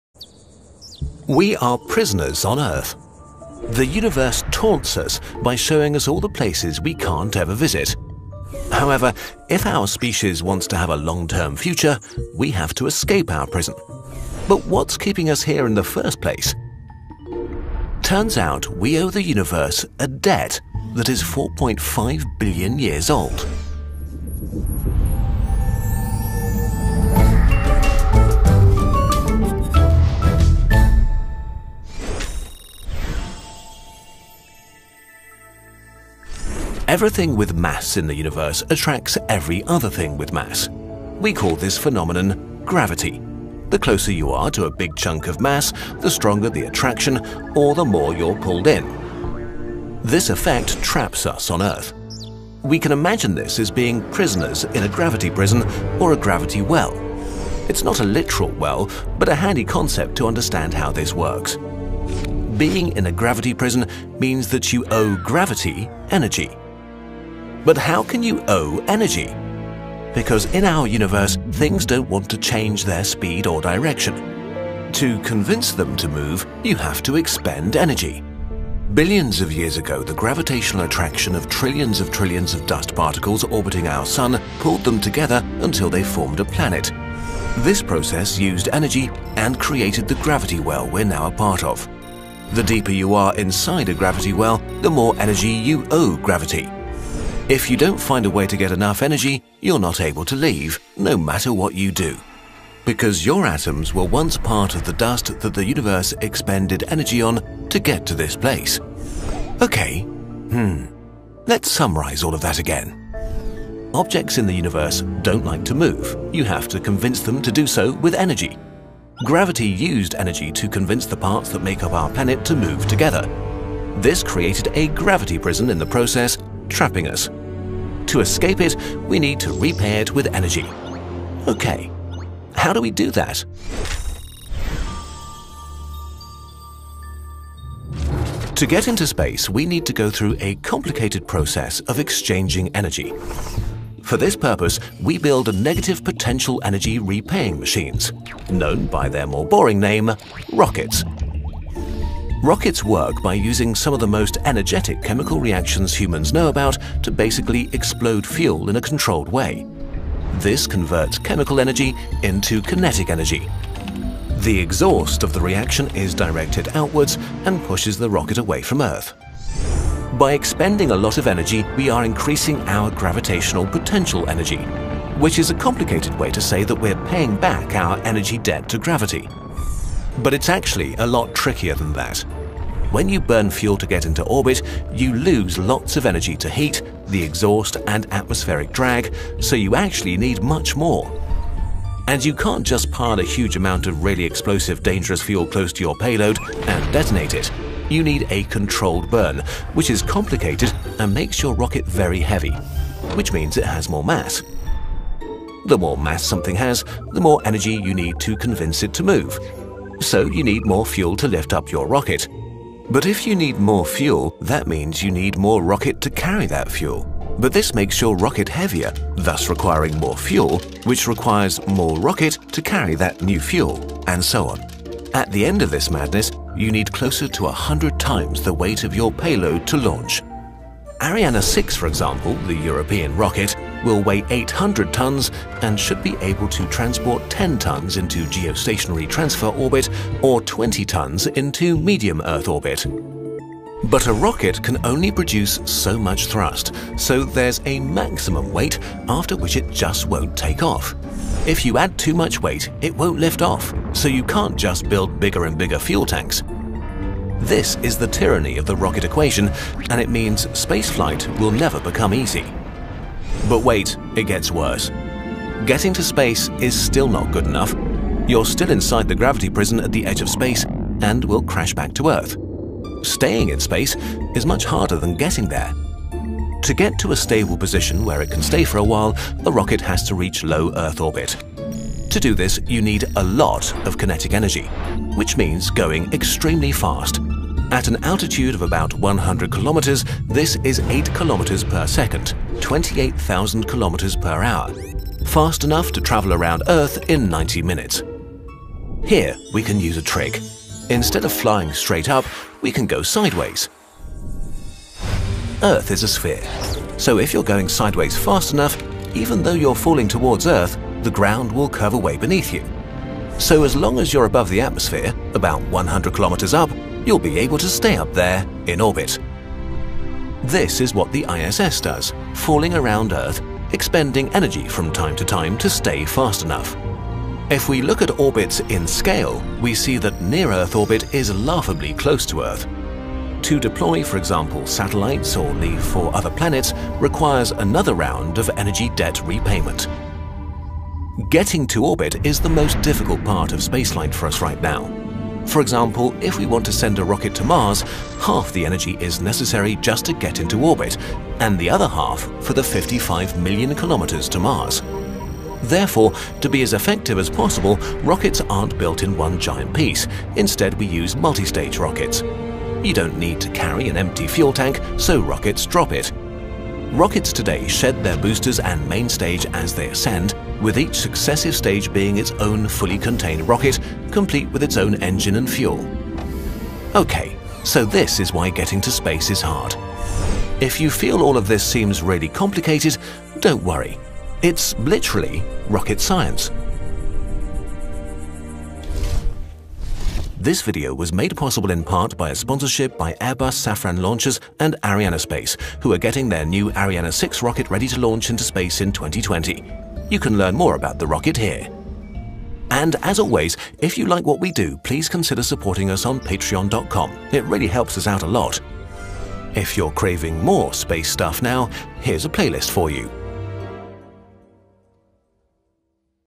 Why Earth Is A Prison and How To Escape It
We are trapped on earth. Controlled by an ancient debt to the universe...